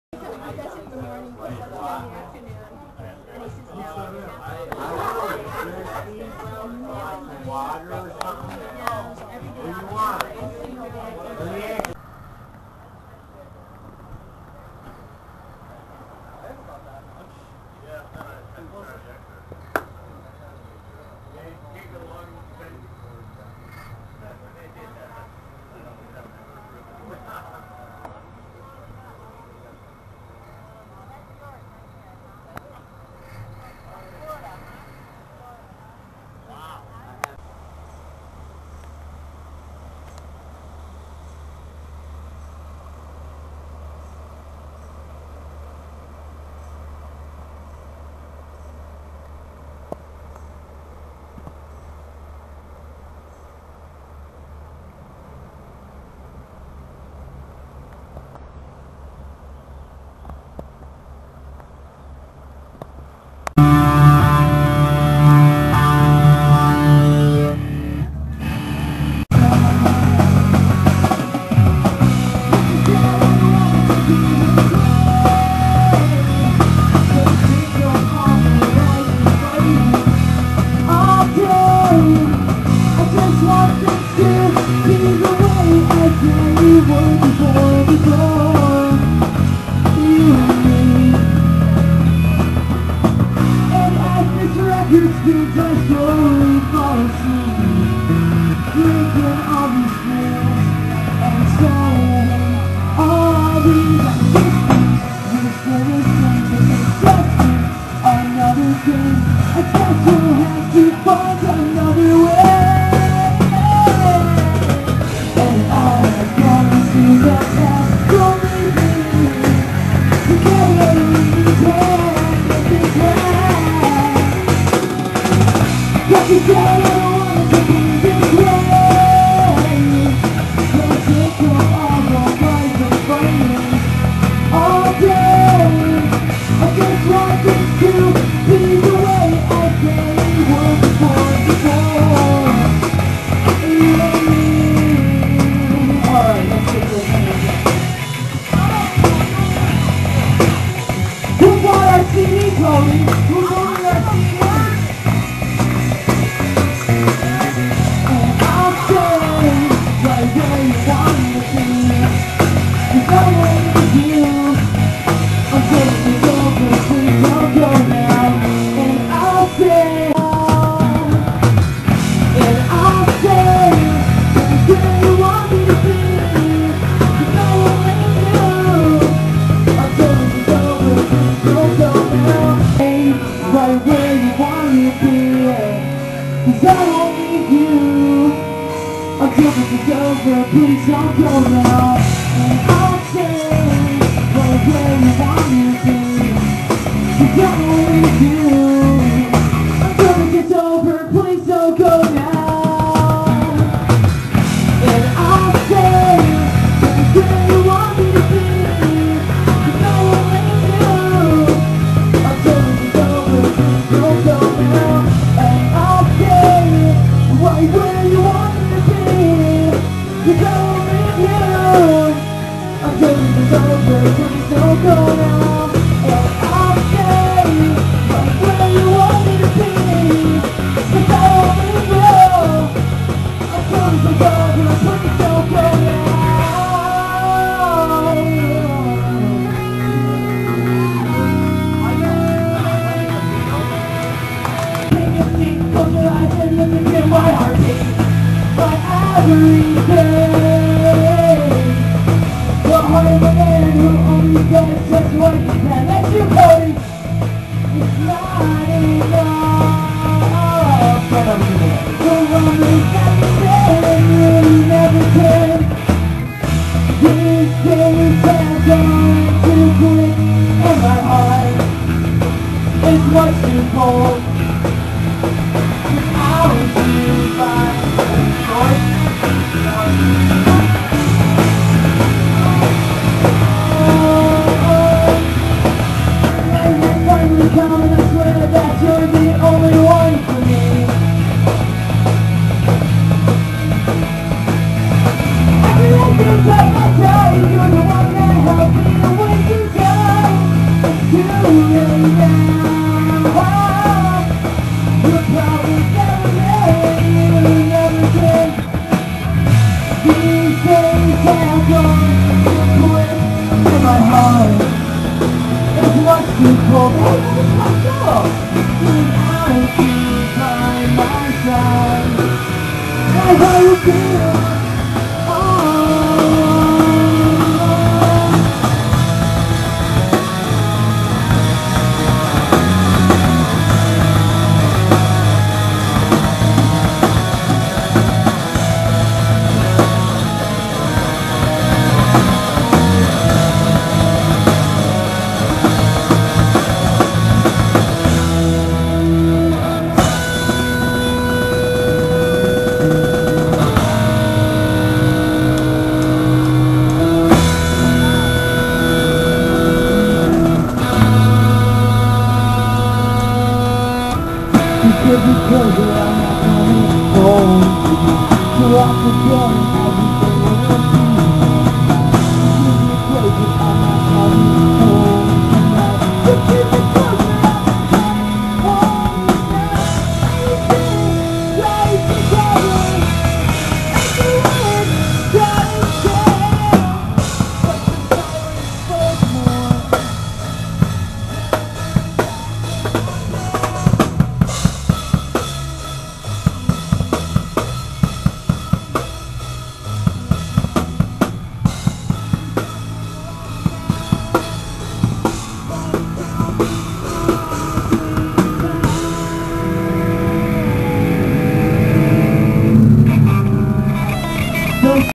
Leta Williams Graduation pt1
July 3rd 2011 Leta Williams graduation party at Ilion Marina. Food, fun, tunes spinning & band playing.